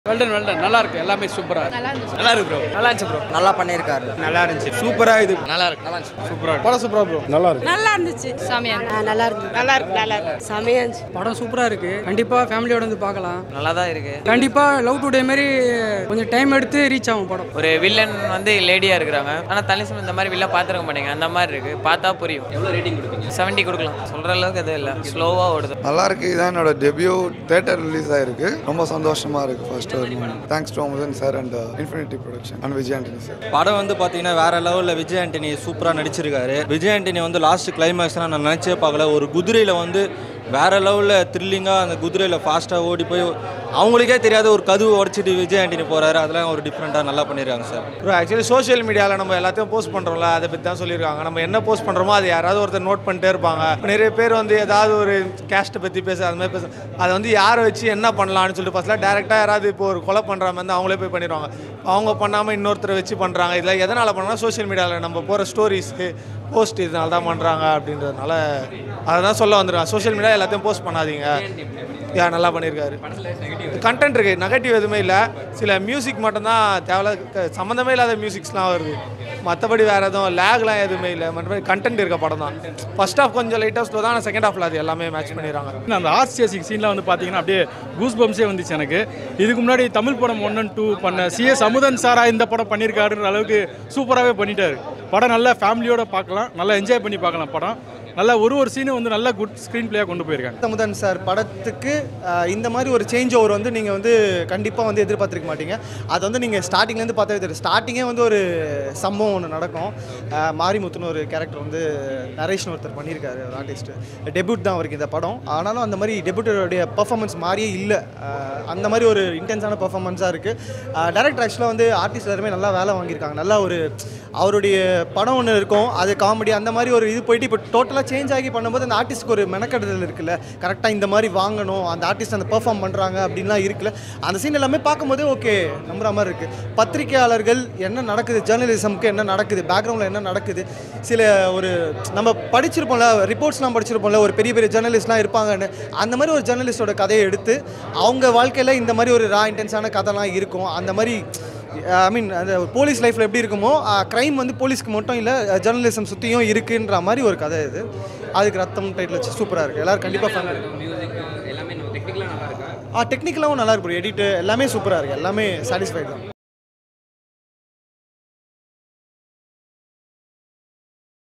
Raththam Public Review | Vijay Antony | Mahima Nambiar | Nandita | CS Amudhan | Provoke Tv
Get alerts when we release any new video. TURN ON THE BELL ICON on the Provoke channel!